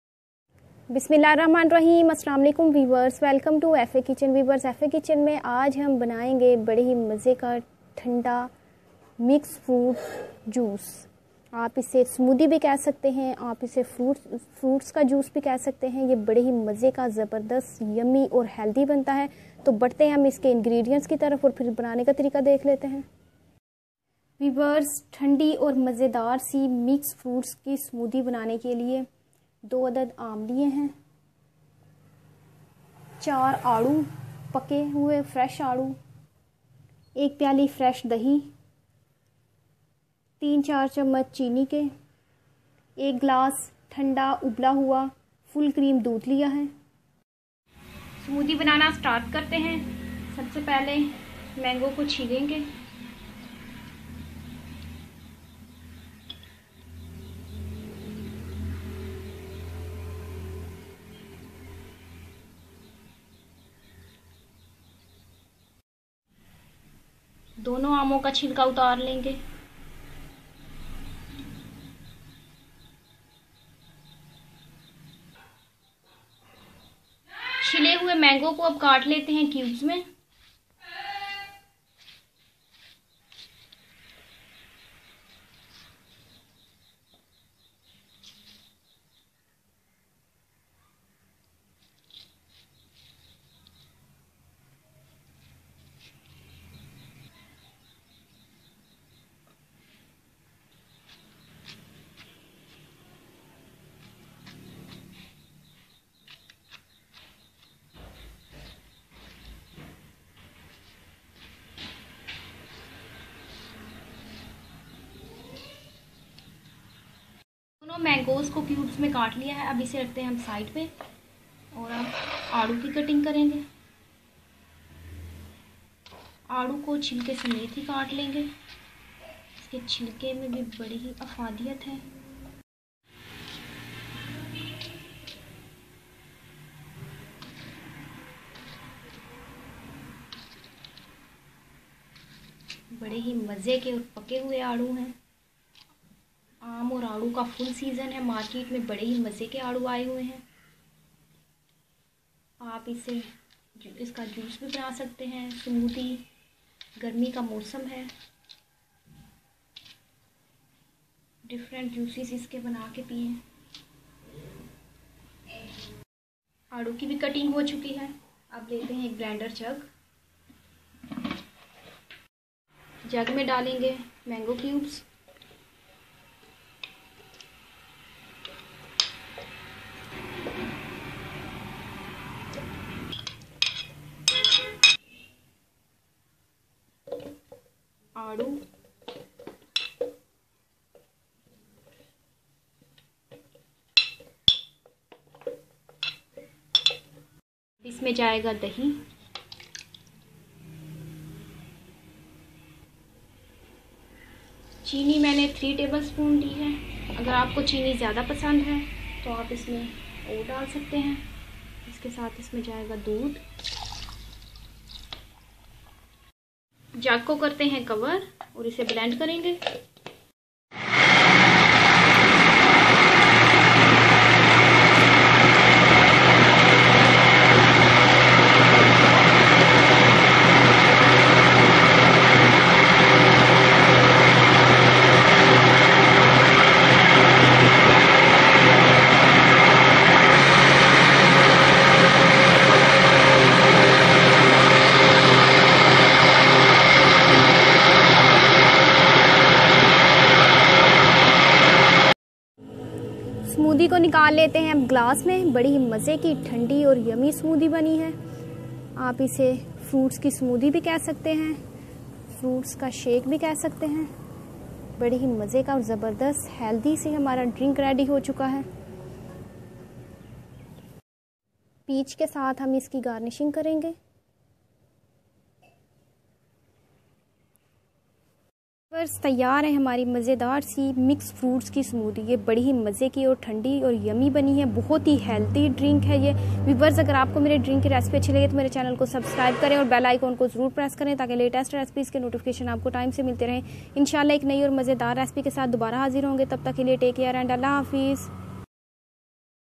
Mix Fruits Smoothie | Fresh Fruits Milkshake | تازہ پھلوں کا ملک شیک | Fresh, healthy &yummy....
How to make Fruits Smoothie or Fresh Fruits Milkshake?
This recipe is very easy to make.

It is very yummy and cool milkshake.
It's all ingredients are very healthy.

It's healthy and incredible taste will give you cool &fresh feelings.

So try it and enjoy it's marvellous taste.